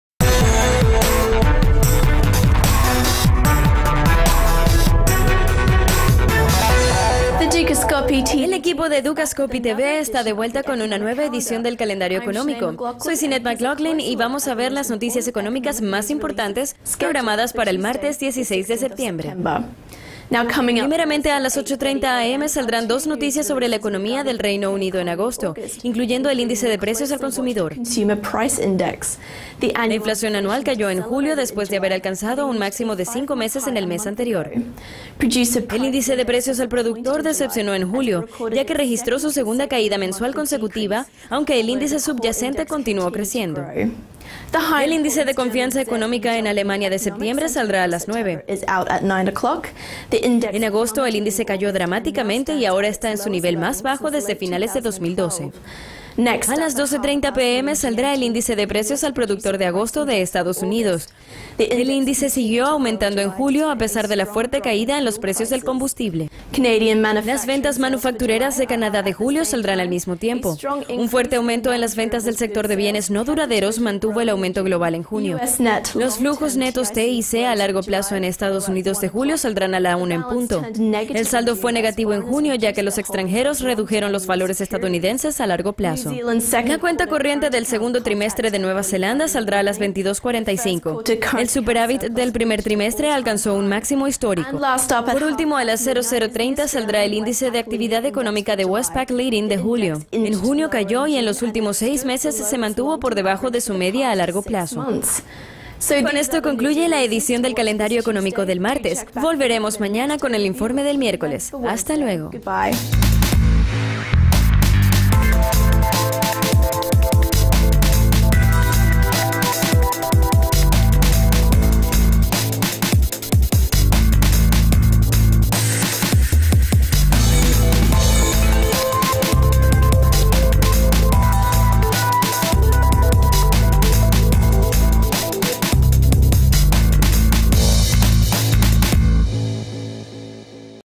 Calendario económico
El equipo de Dukascopy TV está de vuelta con una nueva edición del calendario económico. Soy Sinead
MacLaughlin y vamos a ver las noticias económicas más importantes, programadas para el martes, 16 de septiembre.
Primeramente a las 8:30 AM saldrán dos noticias sobre la economía del Reino Unido en agosto, incluyendo el índice de precios al consumidor. La inflación anual cayó en julio, después de haber alcanzado un máximo de cinco meses el mes anterior.
El índice de precios al productor decepcionó en julio, ya que registró su segunda caída mensual consecutiva, aunque el índice base continuó creciendo.
El índice de confianza económica en Alemania de septiembre, saldrá a las 9. En agosto, el índice cayó dramáticamente y ahora está en su nivel más bajo desde finales de 2012.
A las 12.30 PM saldrá el índice de precios al productor de agosto de EE.UU. El Índice siguió aumentando en julio, a pesar de la fuerte caída en los precios del combustible.
Las ventas manufactureras de Canadá de julio saldrán al mismo tiempo. Un fuerte aumento en las ventas del sector de bienes no duraderos mantuvo el aumento global en junio.
 Los flujos netos TIC a largo plazo en EE.UU de julio saldrán a la 1 en punto. El saldo fue negativo en junio, ya que los extranjeros redujeron los valores estadounidense a largo plazo.
La cuenta corriente del segundo trimestre de Nueva Zelanda saldrá a las 22.45. El superávit del primer trimestre alcanzó a un máximo histórico.
Por último a las 00:30 saldrá el índice de actividad económica de Westpac Leading de julio. En junio, cayó y en los últimos seis meses, se mantuvo por debajo de su media a largo plazo.
Con esto concluye la edición del calendario económico del martes. Volveremos mañana con el informe del miércoles. Hasta luego.